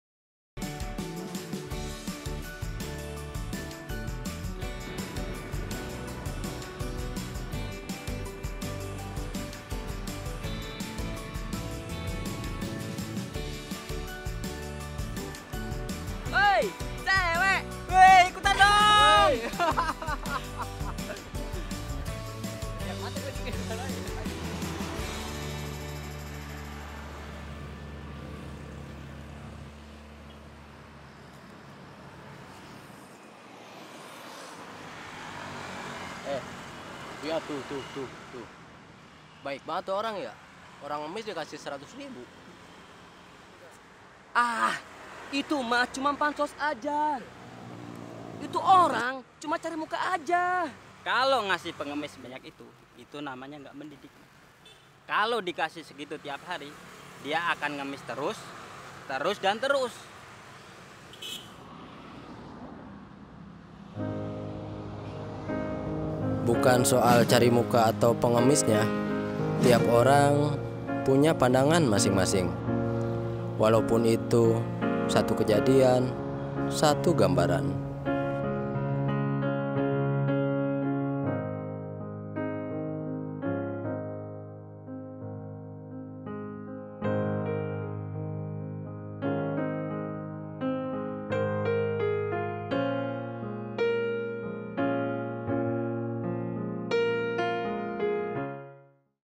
Inspirasi Kehidupan | PERSPEKTIF | Episode 26 | Film Pendek
dalam satu kejadian setiap orang bisa saja berbeda beda dalam pandangannya

Inspirasi Kehidupan | Perspektif | Film Pendek